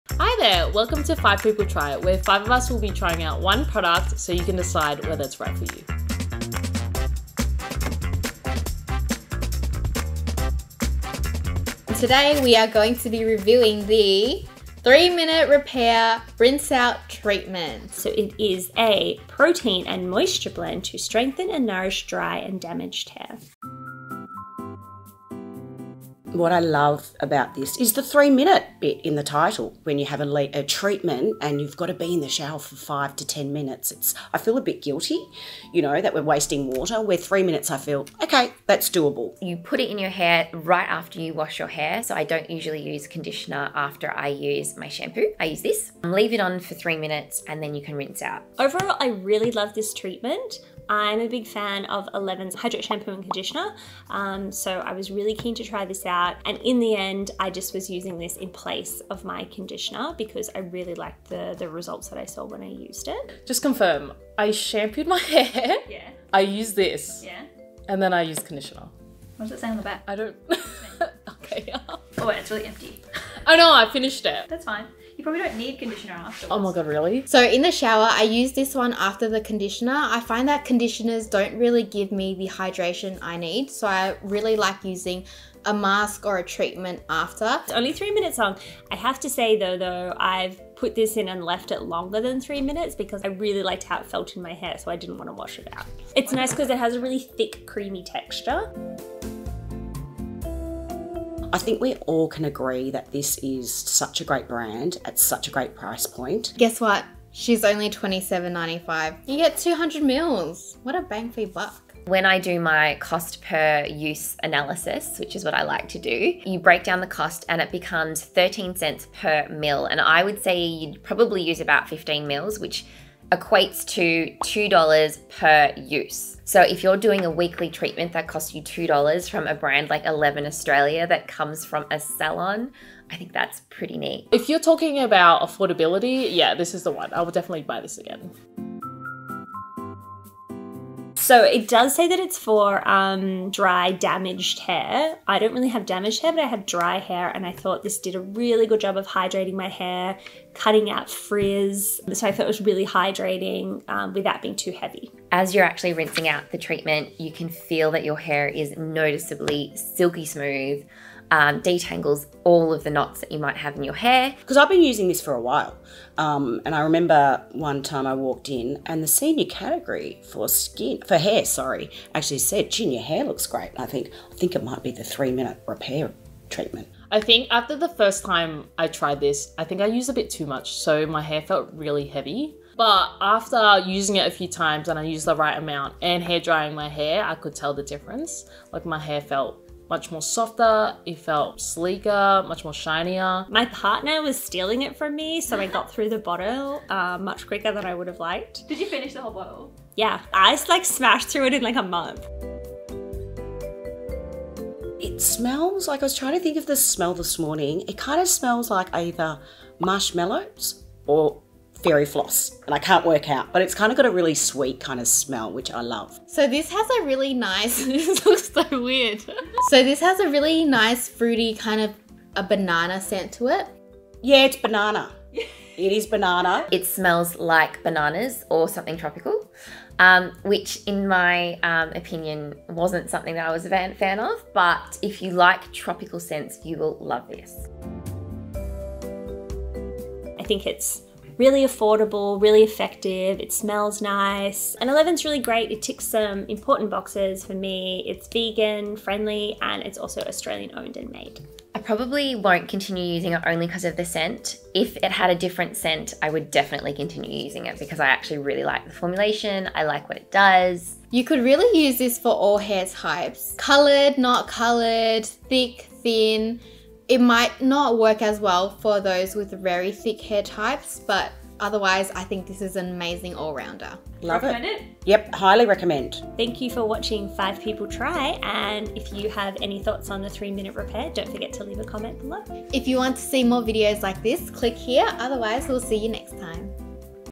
5 People Try & Review ELEVEN 3 Minute Repair Treatment | ELEVEN Reviews
The convenience of a 3 minute hair treatment is too good to pass up - but does this product live up to it's repairing and hydrating claims?

Timestamps:
00:00 Intro
00:24 How to use it
01:39 Cost
02:18 Does it work?
03:22 Scent
03:58 Final thoughts